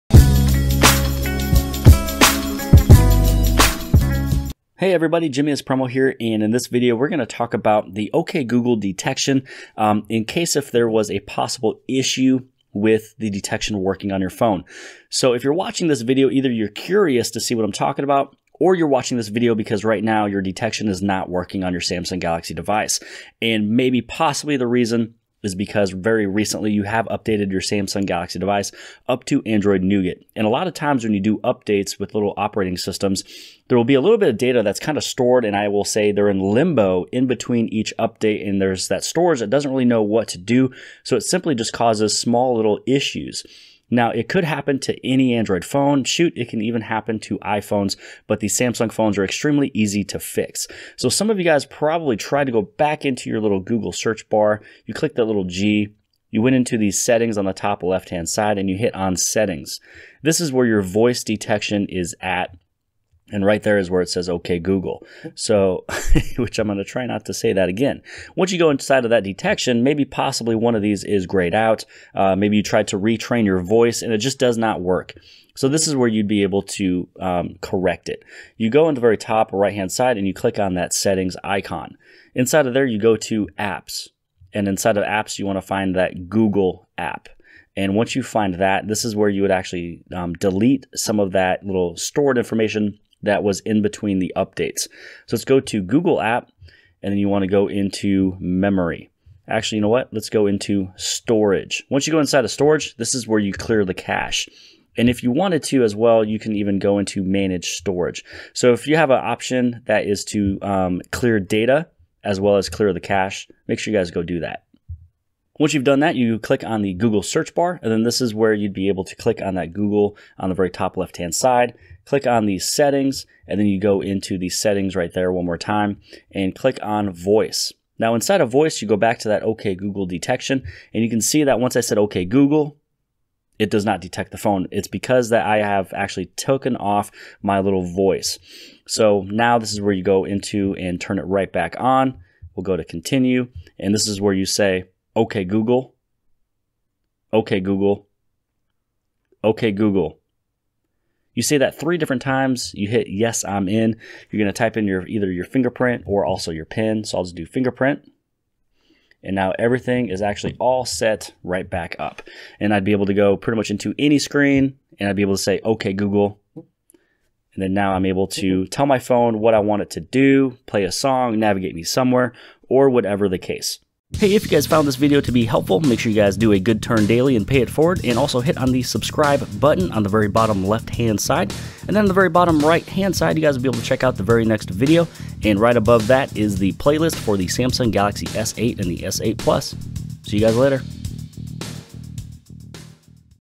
"OK Google" Voice Detection | Issue FIX and Setup
Sometimes when you update the software, some apps have issues. Here is the fix for OK Google voice detection and also setup.